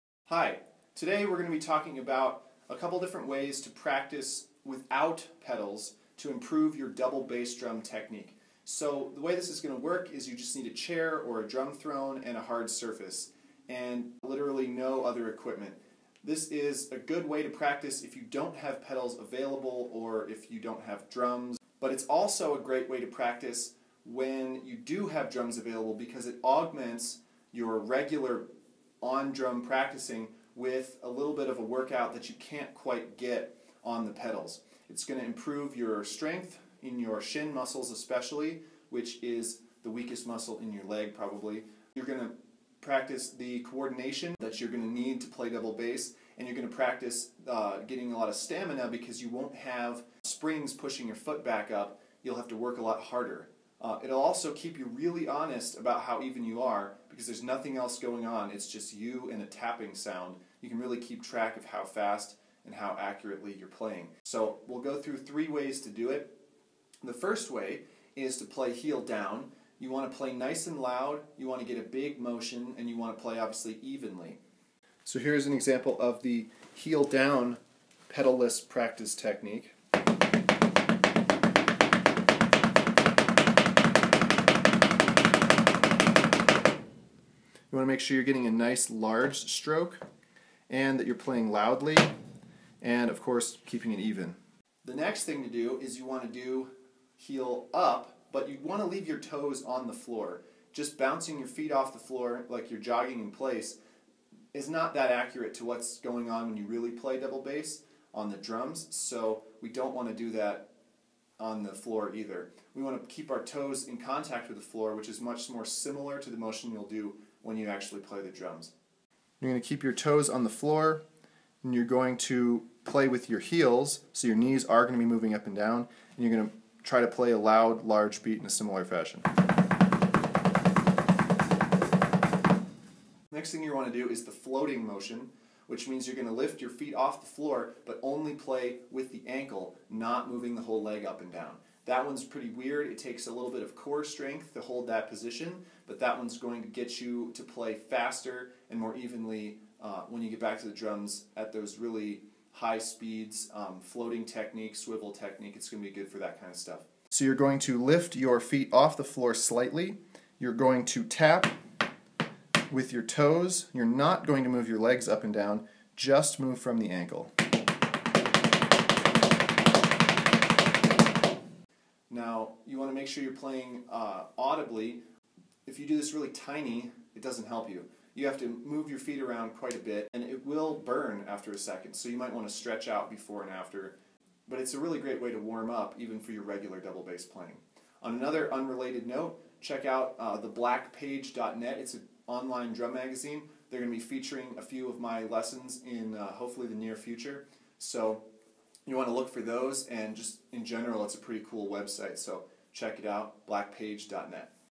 How to Practice Double Bass without Pedals - Floor Exercises
Ryan Alexander Bloom demonstrates how to practice double bass without drums or pedals. These floor-based motions will build strength, stamina, and accuracy anywhere. Even if drums are available to you, these exercises will compliment your usual practice routine and/or make a great warmup.

Get more double bass info and exercises in my book: The Complete Double Bass Drumming Explained, found